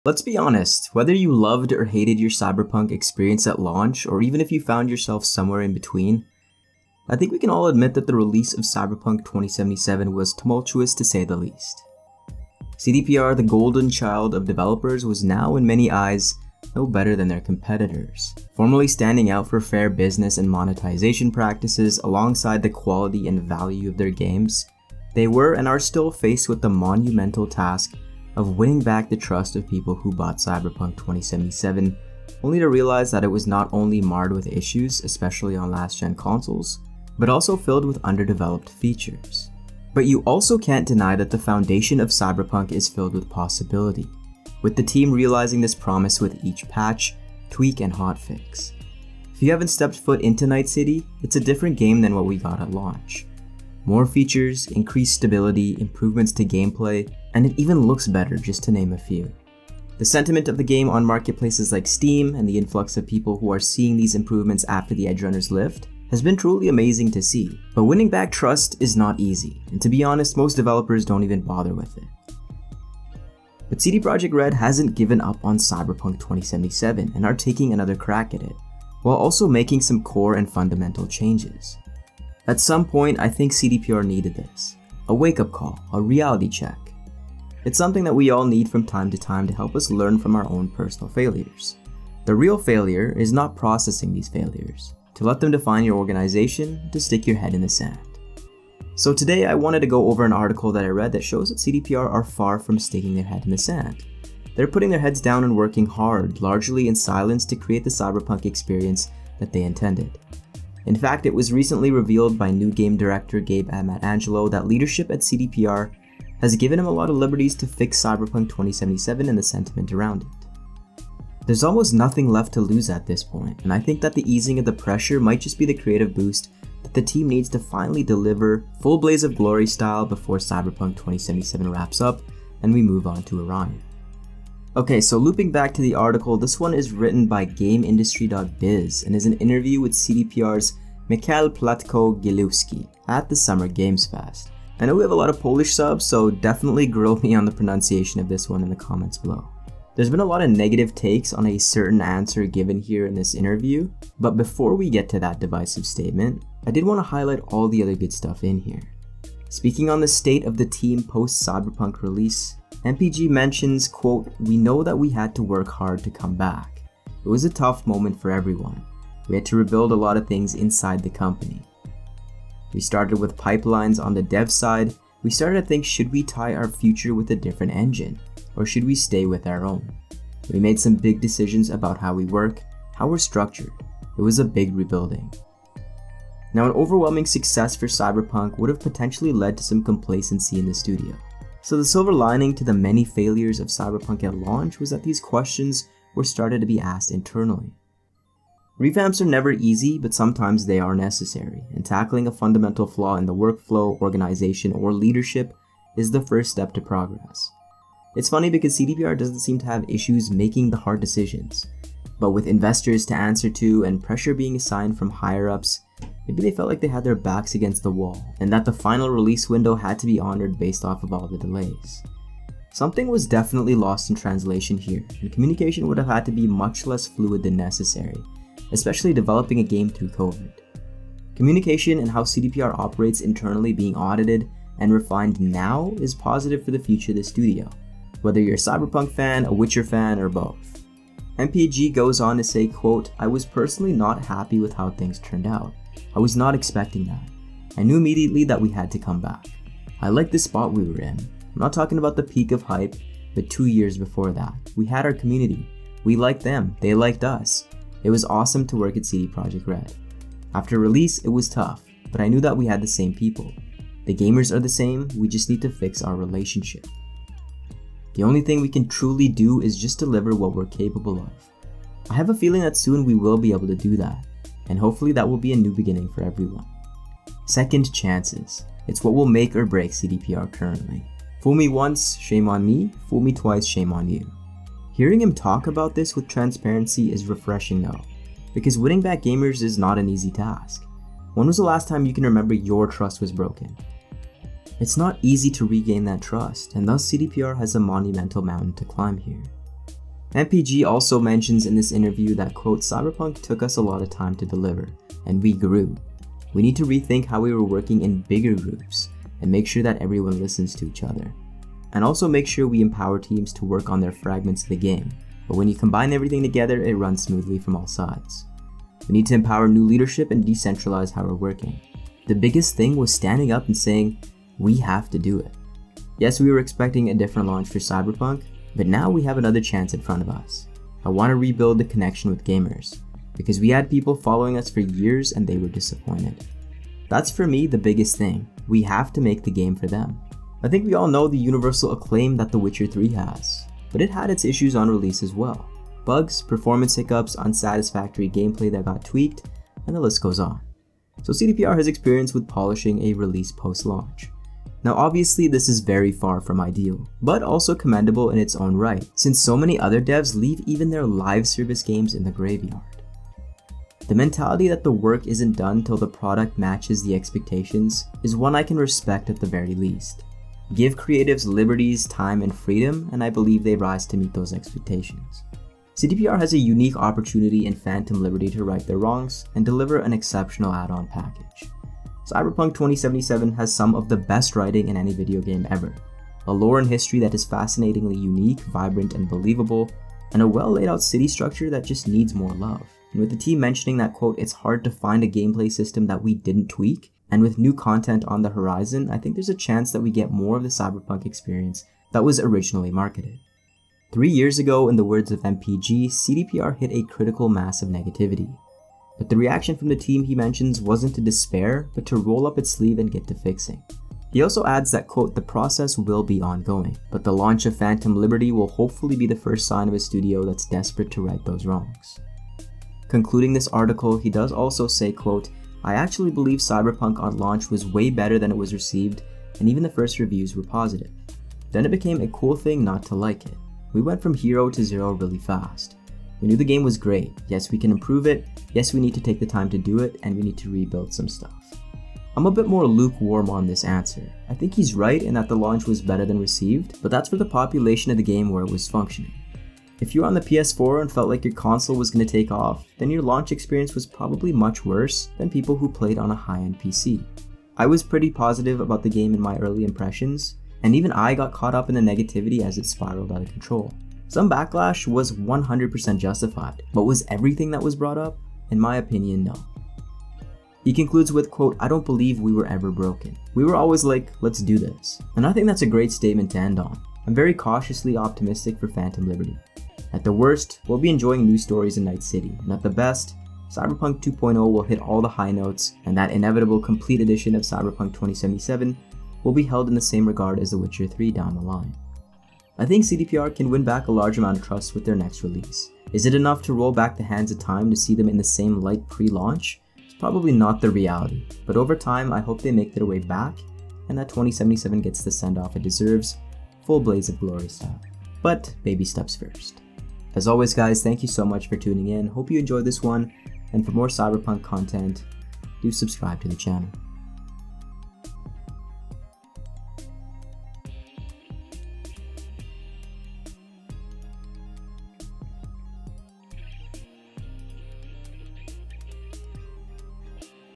CD Projekt Red: "We NEED To Fix Our Relationship With Players"
Can Phantom Liberty turn around the Cyberpunk IP and cement it alongside The Witcher 3 as one of the greats in the CDPR lineup? Some insight into how CD PROJEKT RED is restructuring and retooling, lessons learned, and winning back trust with Phantom Liberty is to be discussed today based on an article from GamesIndustry.Biz